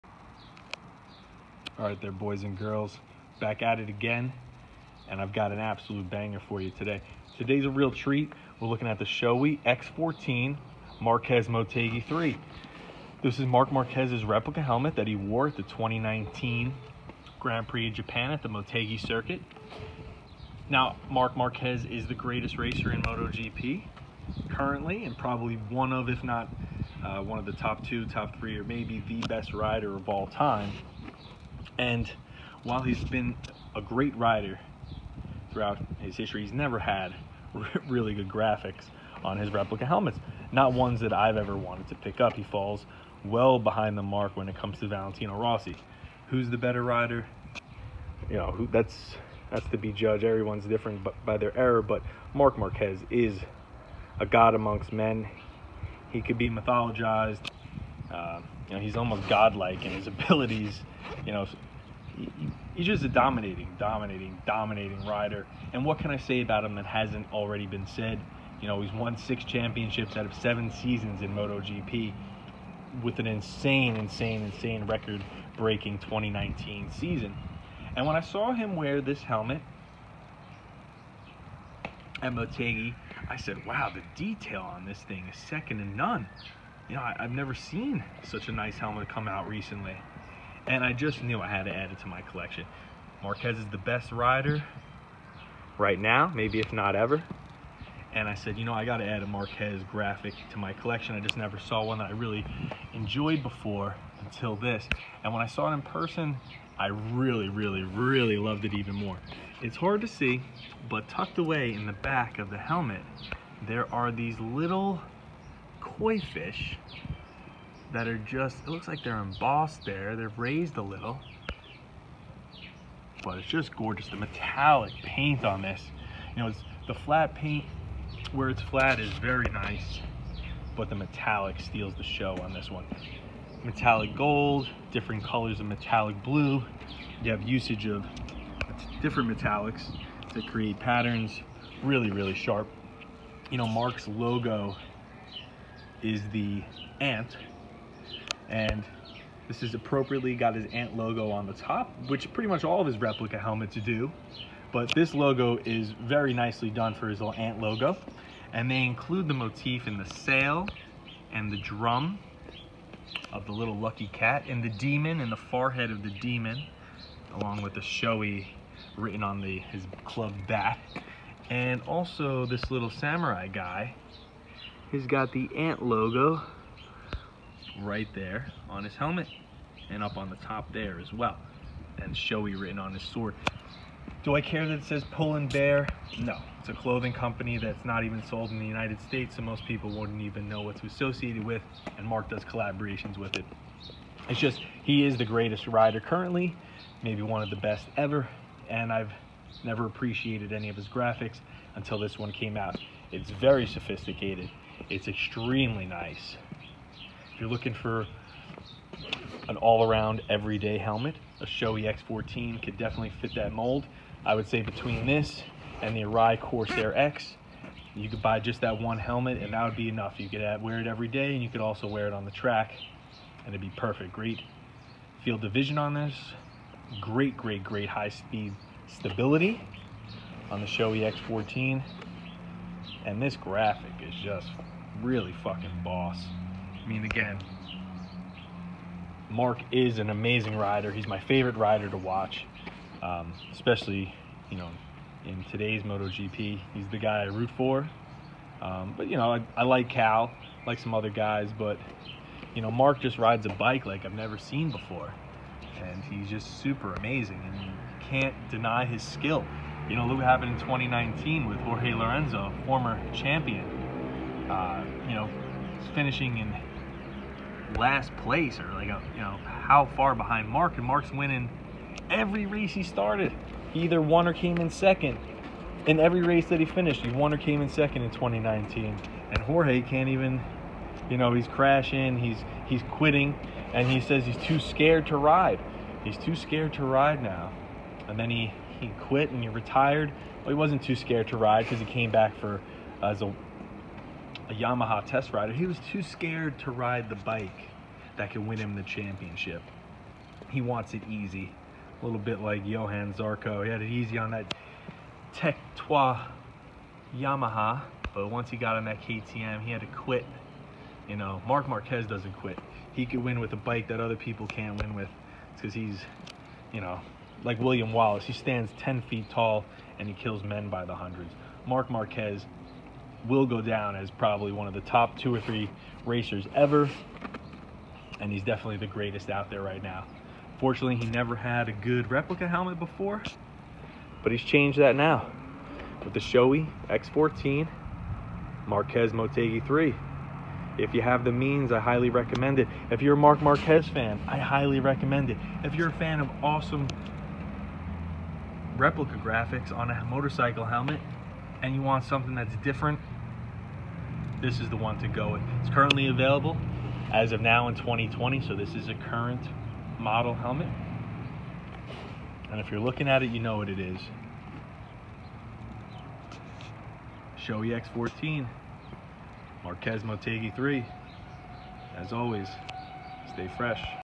Shoei X-14 Marquez Motegi 3 - The Best Rider in the World’s Best Replica Helmet
There aren’t any superlatives I can say about Marc Marquez that haven’t been said already. Dude is already one of, if not THE greatest motorcycle racer of all time. And that’s not hyperbole.

Stepping up to MotoGP in 2013, Marquez won in his rookie season. He followed it up in 2014 with another MotoGP championship. Though he lost the championship in 2015, he hasn’t lost since. 6 championships in 7 seasons in the premiere class. Marc Marquez is a fucking animal.

But one thing Mar Marquez never did well, was helmet graphics. His replica lids have always left me unimpressed. That was until the 2019 Japan GP at Motegi, where I saw Marc rocking this helmet. When I saw Shoei was releasing it, I decided to pick it up for the collection. Can’t have a collection of motorcycle replica helmets, without the helmet of arguably the best ever.

I jumped on this helmet, and I am glad I did. In person, this helmet is freaking AMAZING. The amount of nuance, and level of detail is staggering. The metallic paint is phenomenal, with different hues of gold and blue. I really dig the demon & samurai characters, along with the lucky cat. This is also the best use of Marquez’s signature “Ant” logo on any of his replicas.

If you are a Marc Marquez fan, THIS is the helmet you should have in your collection. If you are just in the market for a helmet, the Shoei X-14 is one of the best all-around lids available. Great for the track, and you can wear it all day on the street. If you have the means, I highly recommend adding this to your collection.

As always, stay fresh.